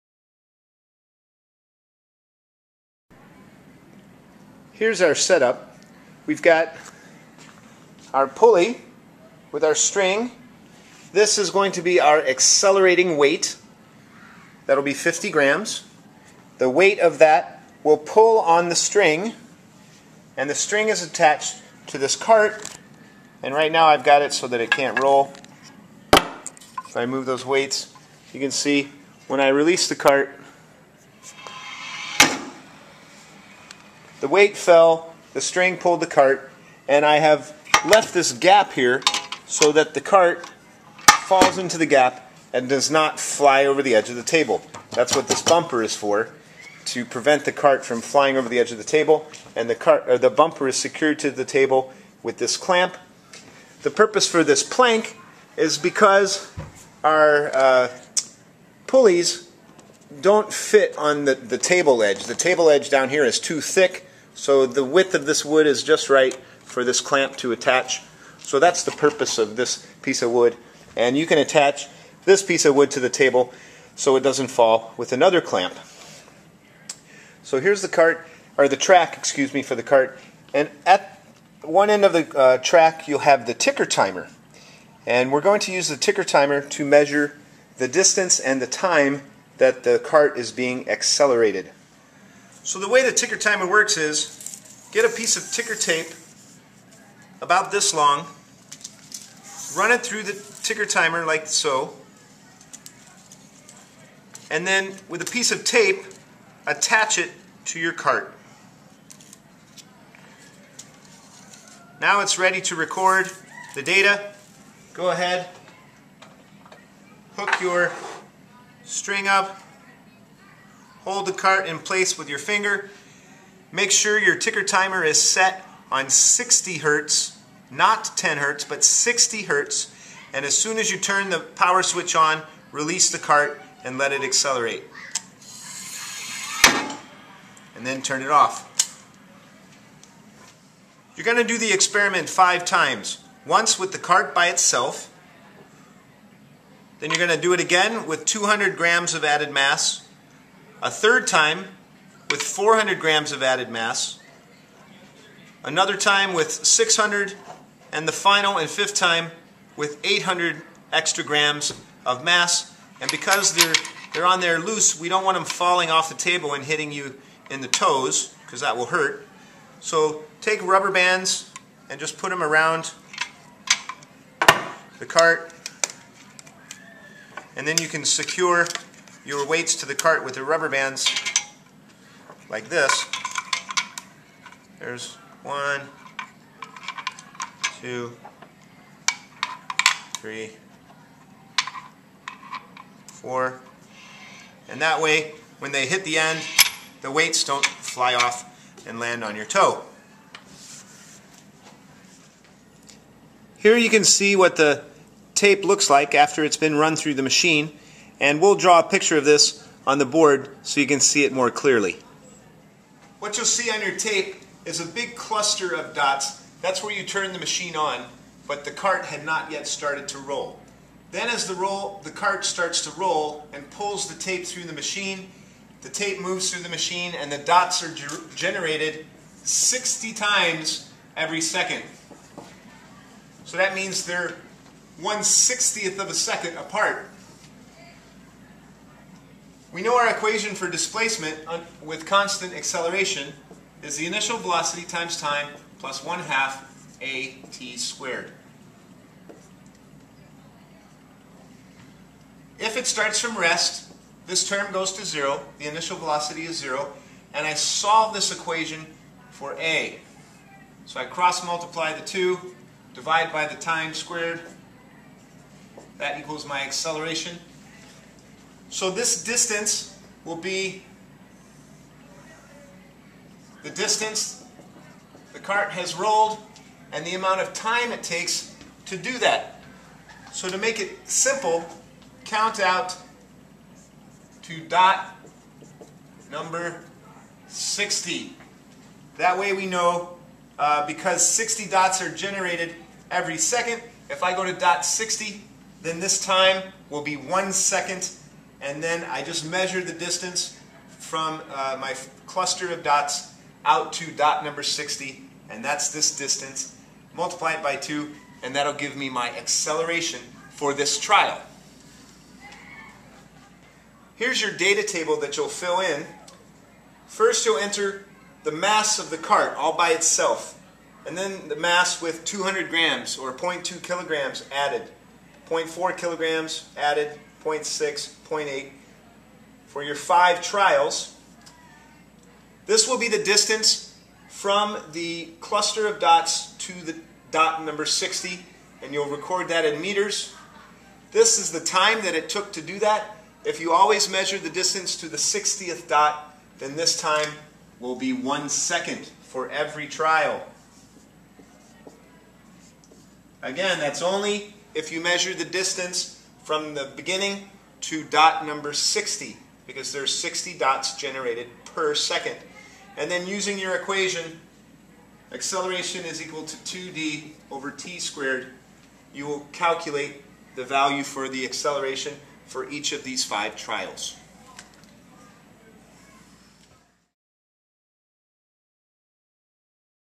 Newton's 2nd Law Accelerating Carts
A physics lab for demonstrating the inverse relationship between mass and acceleration.